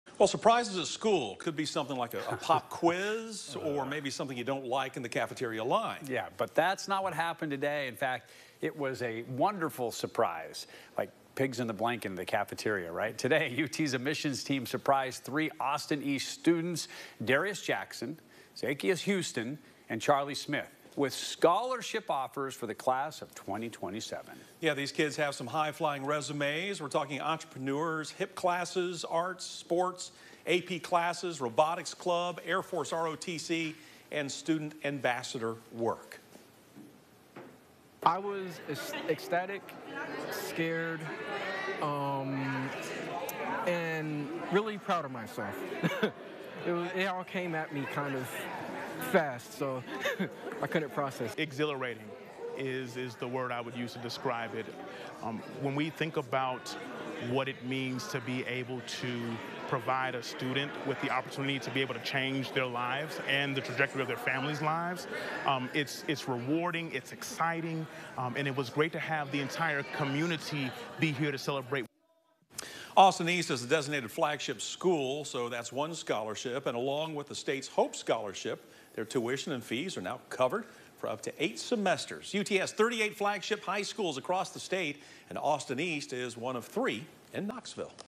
Scholarship surprise at Austin-East
Darius Jackson, Zakeyous Houston and Charlie Smith received a surprise visit from the University of Tennessee, Knoxville’s admissions team on Wednesday.